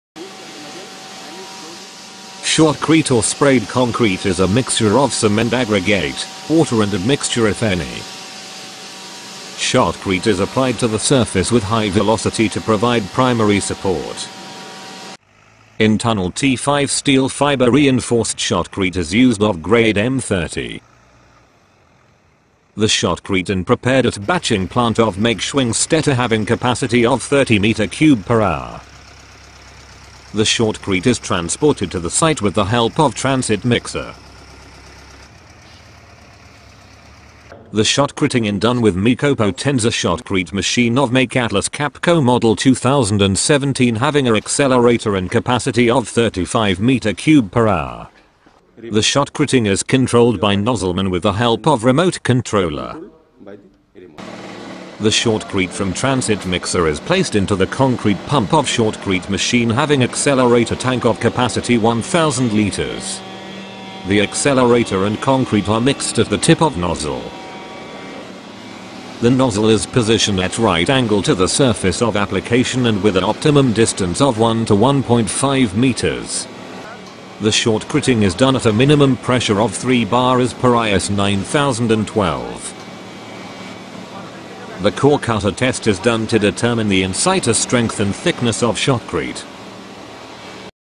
Shotcreting as primary support in tunnels
The video shows the details of shotcreting or sprayed concrete in tunnels for providing primary support.